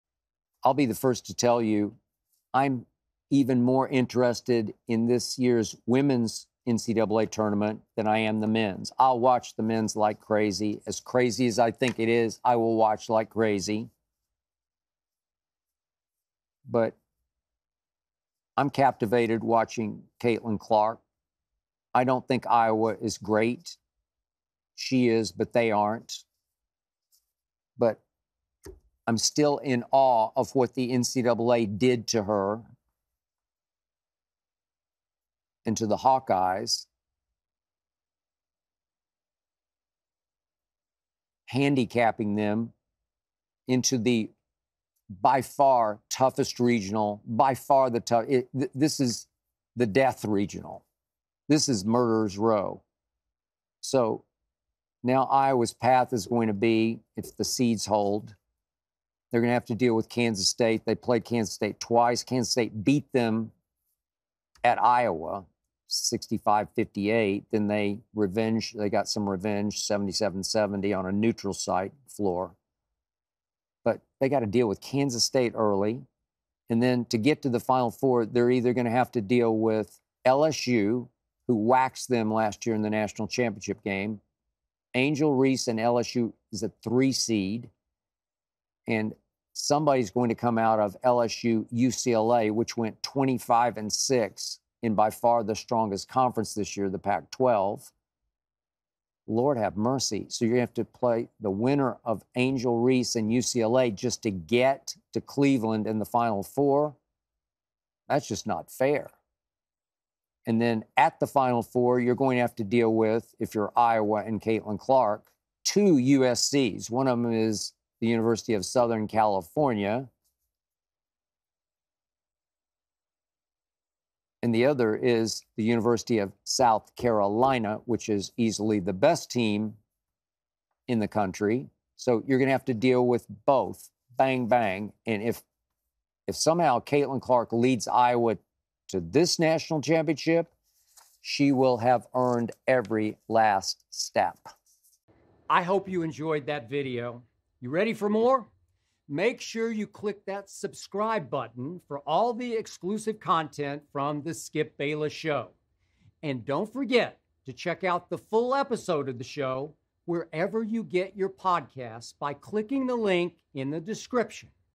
Skip says he's eager to watch Caitlin Clark, "more interested in this year's Women's NCAA tourney"
Skip Bayless says that he is "more interested in this year's Women's NCAA tournament than he am the Men's," then explains why he is excited to watch Iowa star Caitlin Clark.